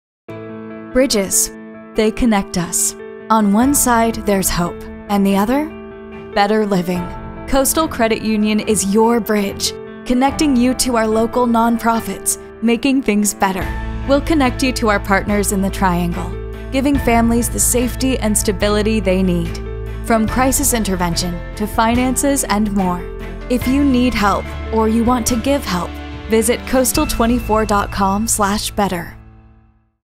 Bridging Crisis Intervention
Coastal Credit Union is your bridge, connecting you to our local non-profits, making things better. We’ll connect you to our partners in the Triangle, giving families the safety and stability they need. From crisis intervention, to finances and more, if you need help, or you want to give help,